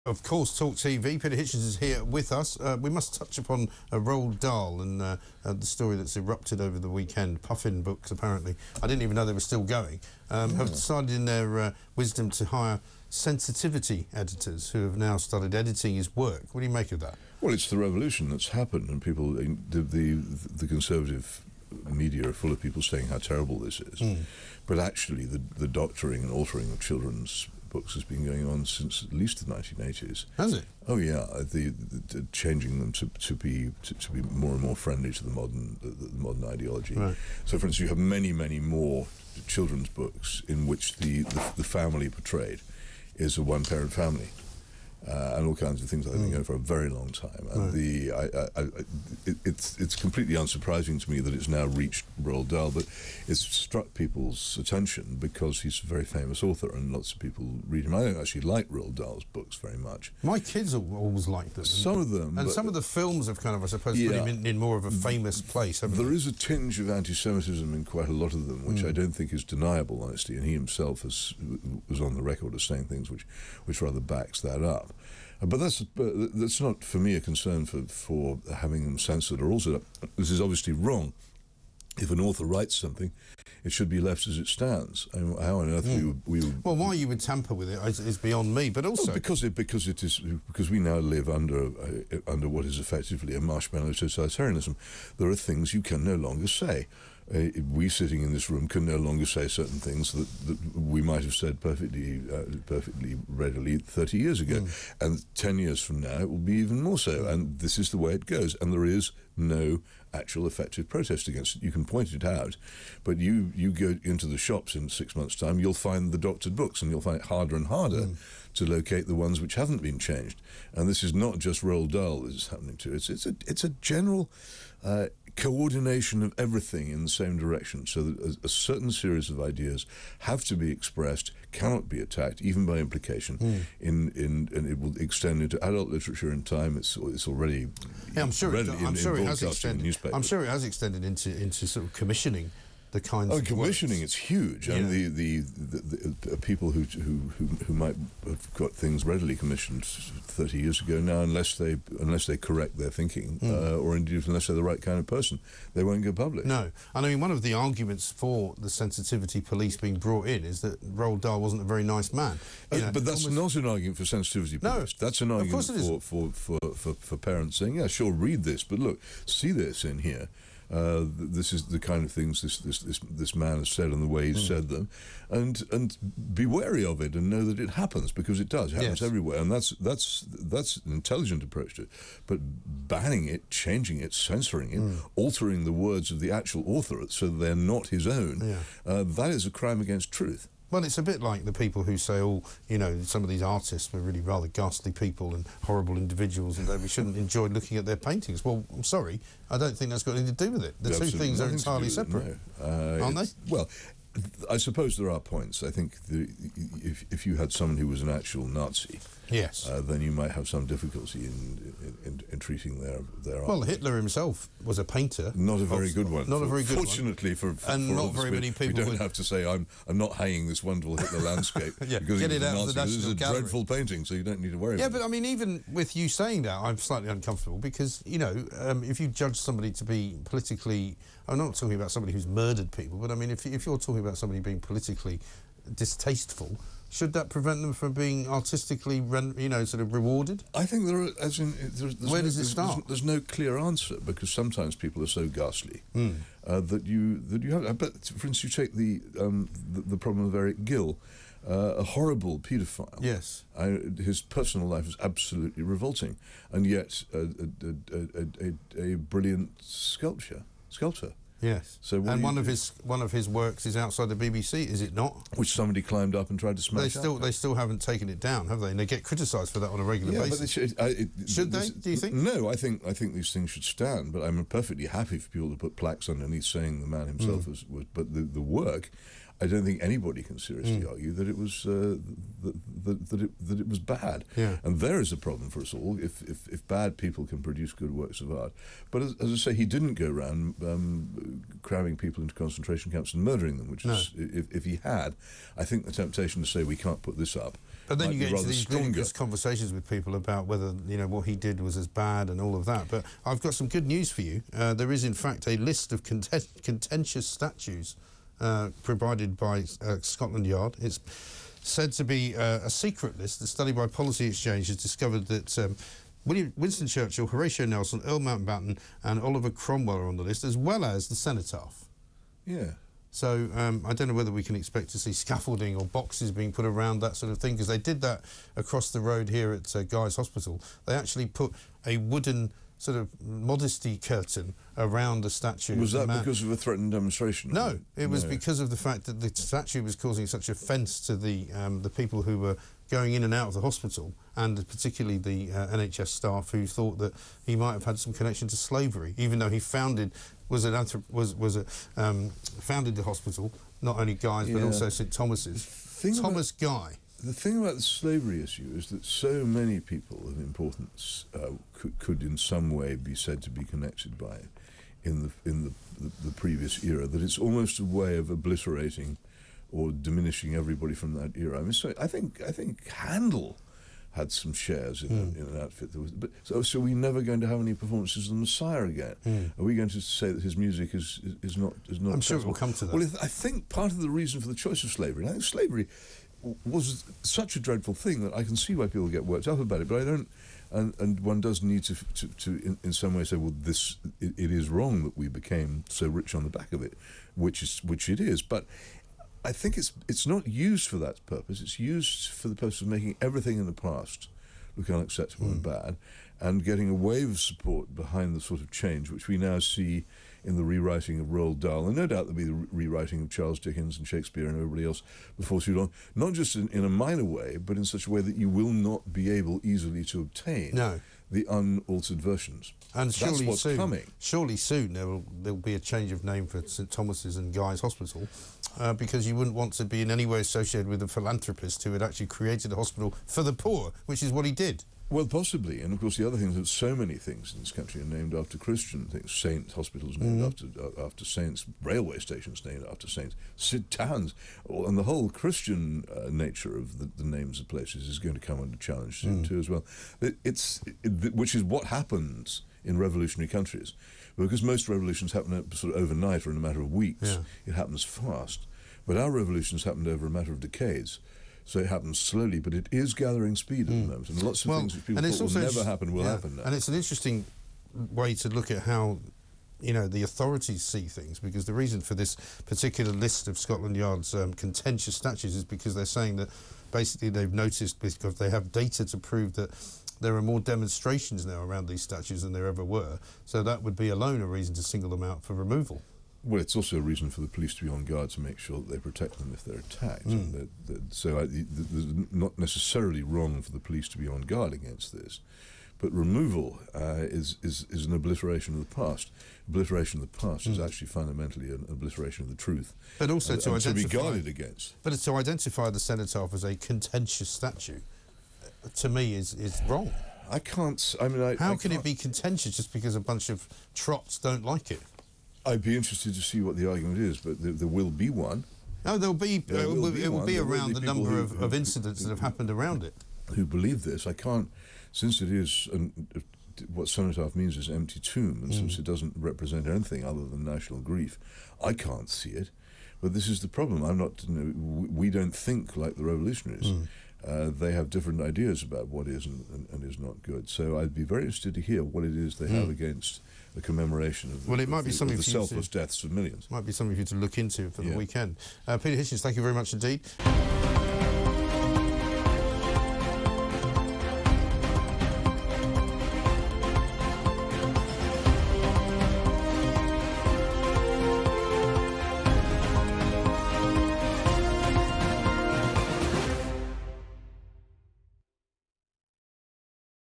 Roald Dahl book edits: Peter Hitchens says past being 'OBLITERATED'
Journalist Peter Hitchens tells TalkTV the doctoring of children's books has been going on since the 1980s, and the nation was effectively living under a totalitarian state where "there are things you can no longer say", adding he thought the past was being obliterated.

Prime Minister Rishi Sunak has also condemned the rewriting of Roald Dahl’s books after the latest editions were edited to remove potentially offensive language.

Author Salman Rushdie claimed that edits to Dahl’s books are “absurd censorship” while others said the publishers should be ashamed.

Books by the much-loved but controversial children’s author have been rewritten to reportedly cater for the sensitivities of modern audiences.